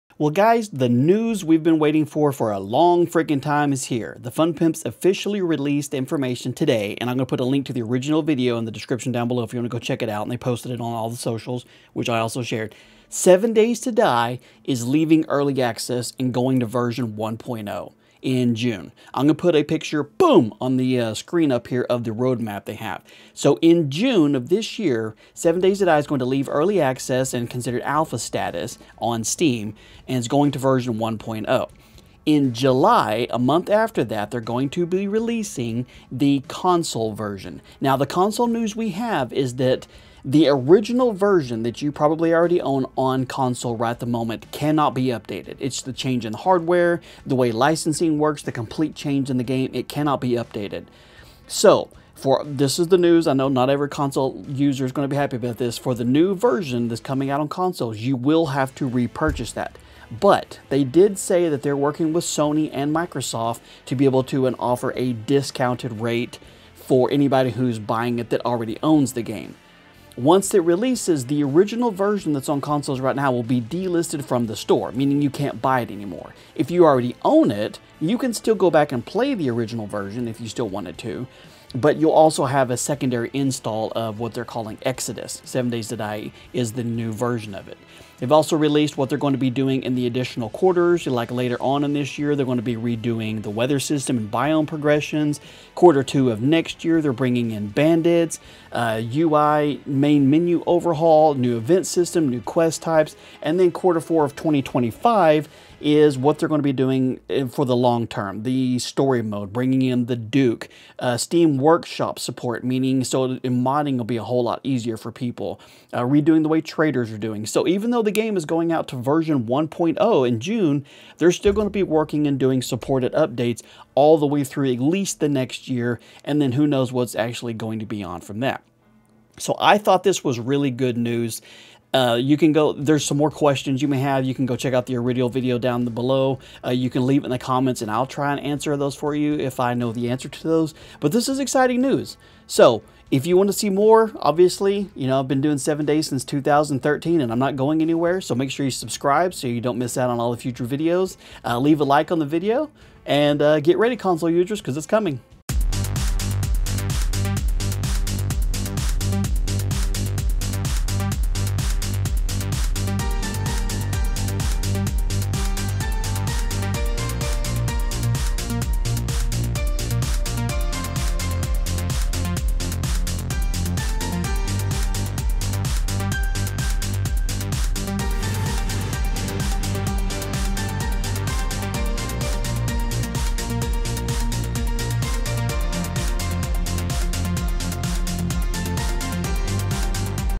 7 Days to Die Console Release Info!! Plus Leaving Early Access!
7 Days To Die is leaving early access into Exodus 1.0. This also means that the Consoles are getting updated! The news we have so far!